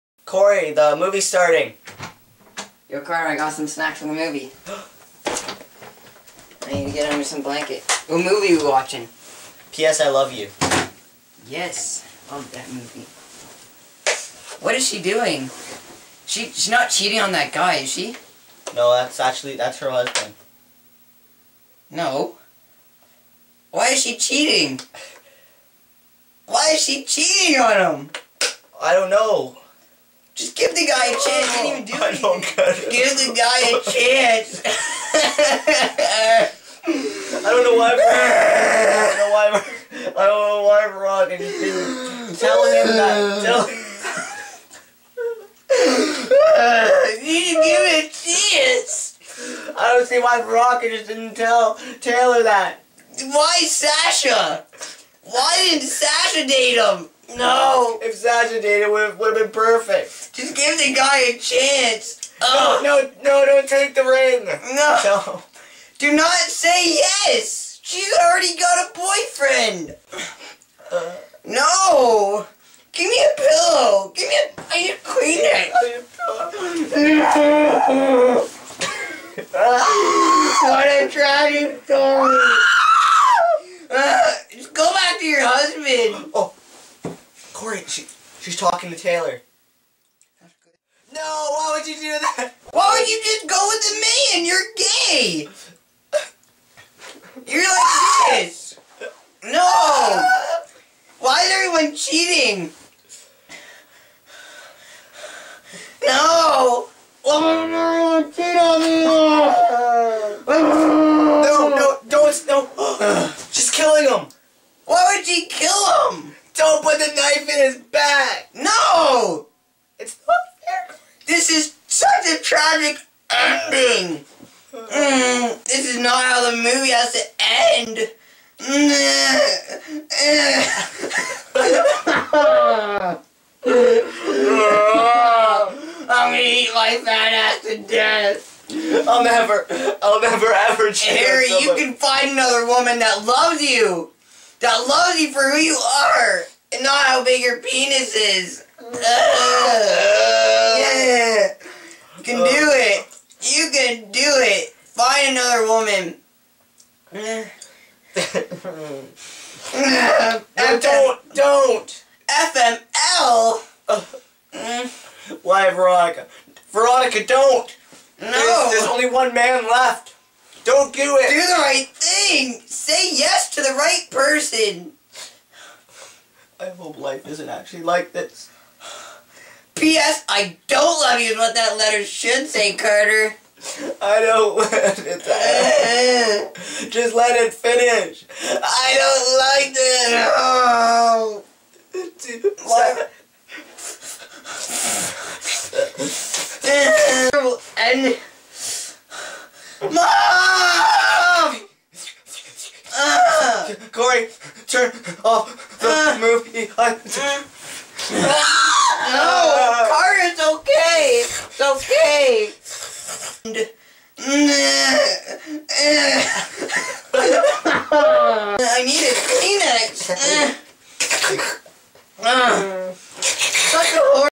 Carter & Cory watch a chick flick.
carter and cory get all emotional over a movie. ( not really p.s i love you i couldnt think of a name.)